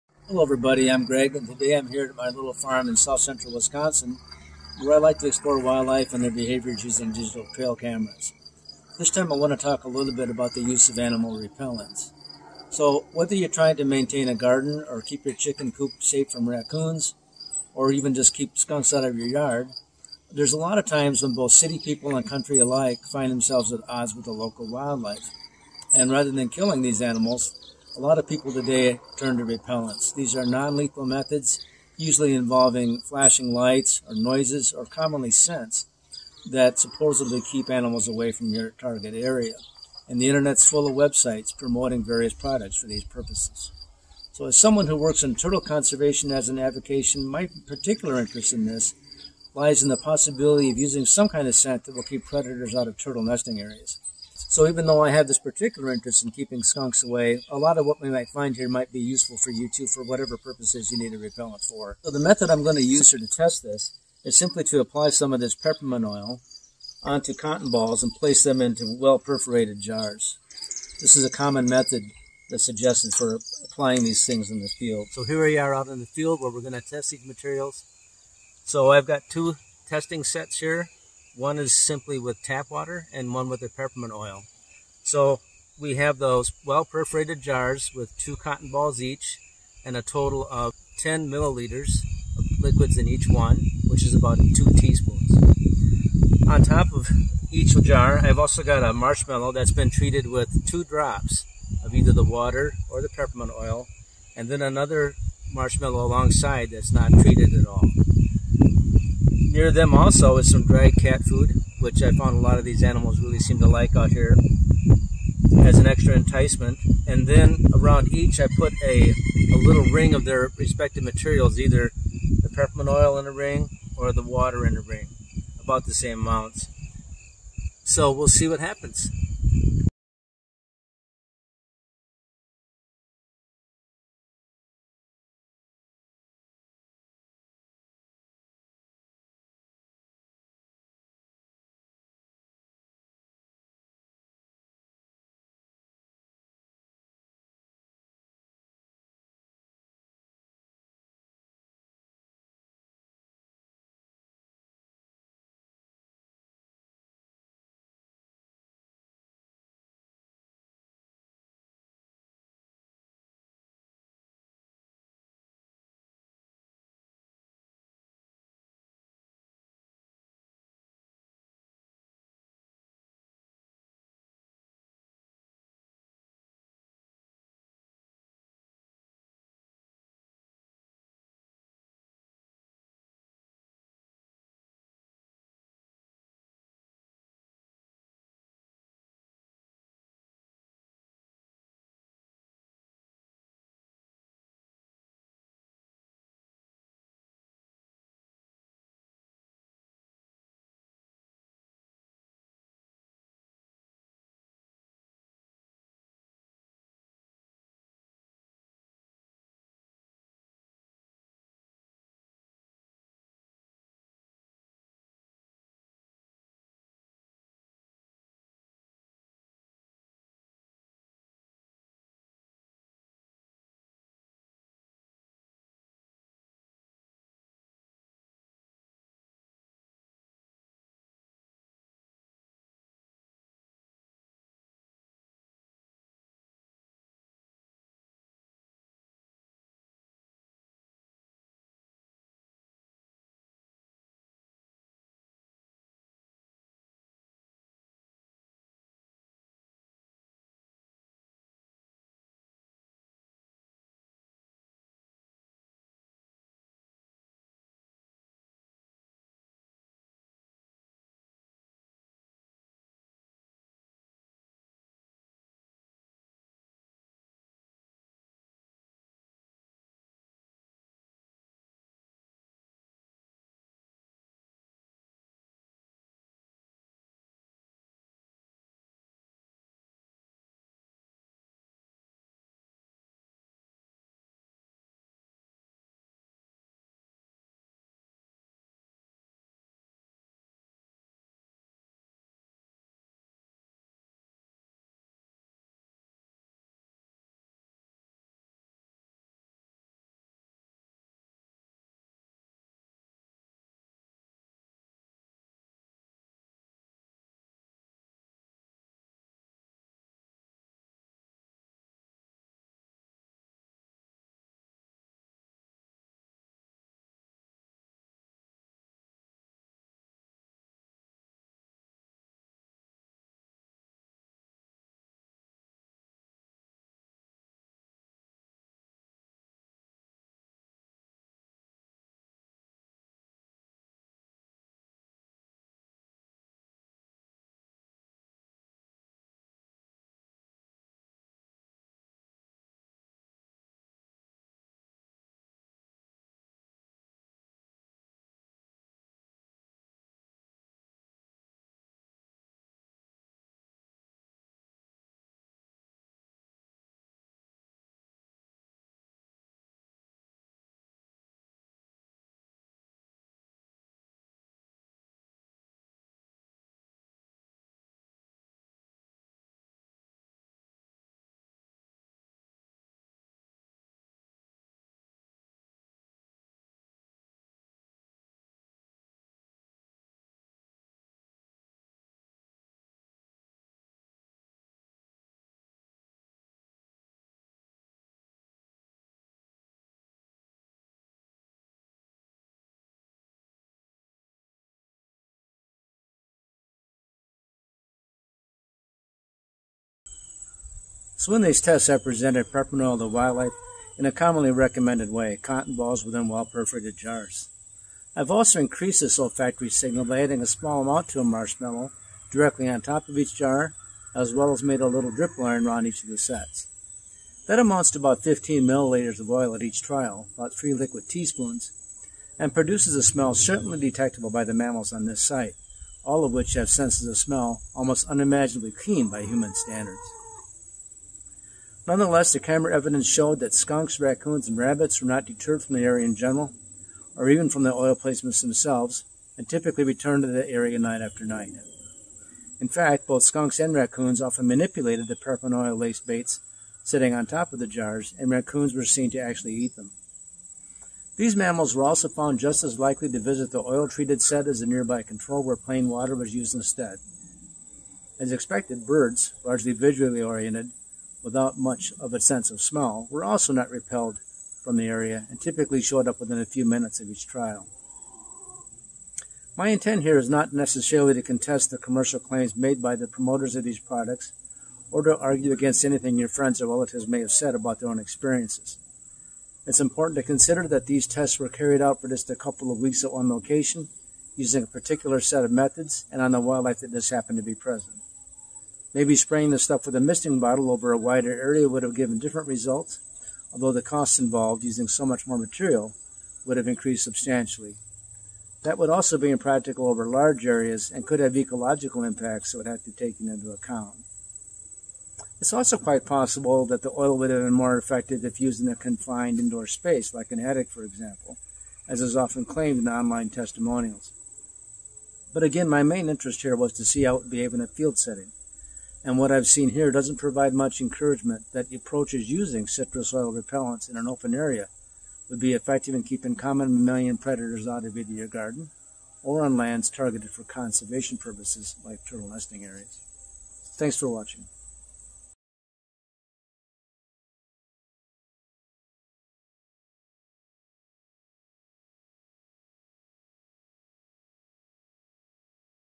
Peppermint oil as skunk and raccoon repellent
Peppermint oil was tested as a mammal repellant in southwest Wisconsin, 2023. It was found to be ineffective in an outdoor setting against raccoons and skunks, as both of these mammals repeatedly came into direct contact with an oil-treated experimental set-up (approximately 15 mls of oil) over the course of several days. These results suggest that citrus oils are not likely to be of much value in keeping mid-sized mammalian predators away from areas in the field, including those targeted for conservation purposes.